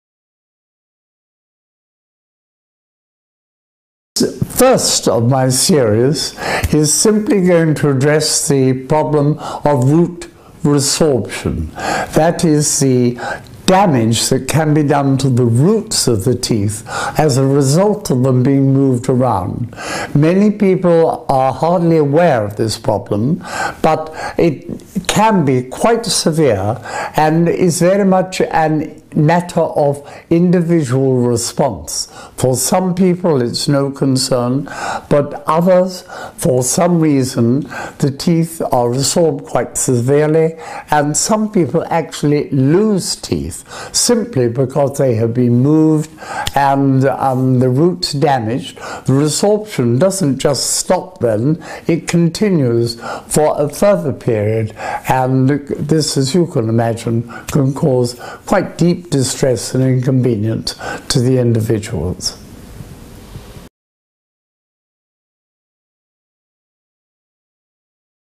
変わるべき歯科矯正：歯根吸収は問題か？
記念すべき第50回動画は、「変わるべき歯科矯正：歯根吸収は問題か？」というタイトルでお送りしています。ジョン・ミュー先生が現在の歯科矯正の問題について取り上げているシリーズの第1回目です。